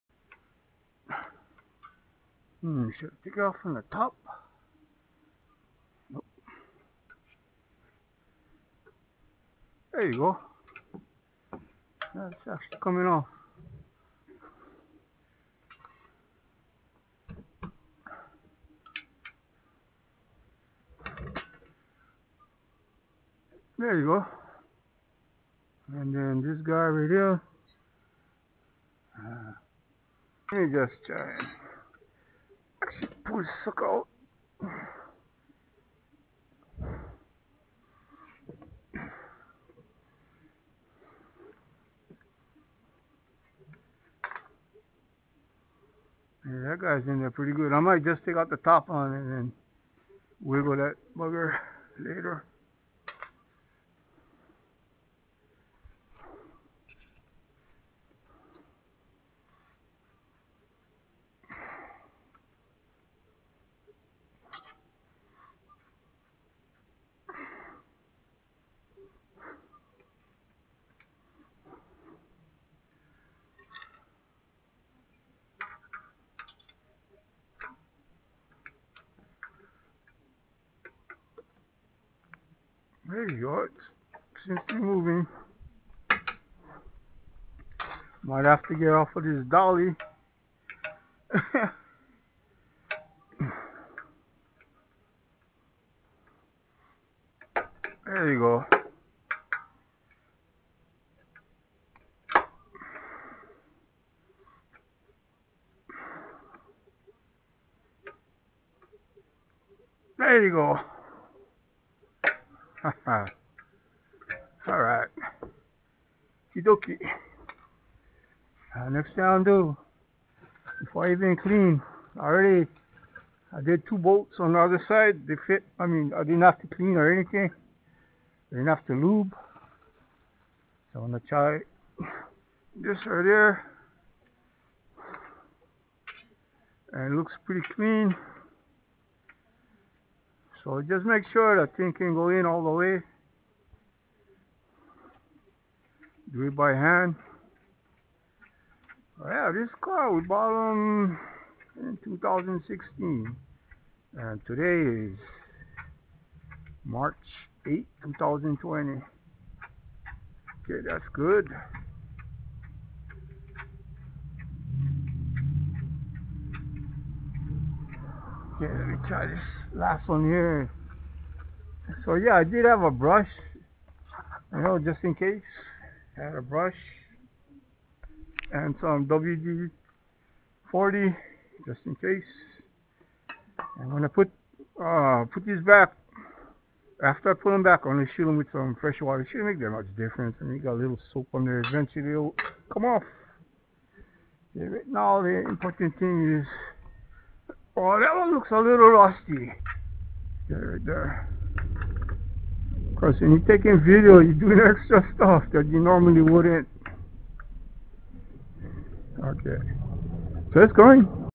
Trailer Hitch Removing Rubber Exhaust Hangers and Test Fitting Hitch Bolts
Contact a Competent Mechanic for this install :)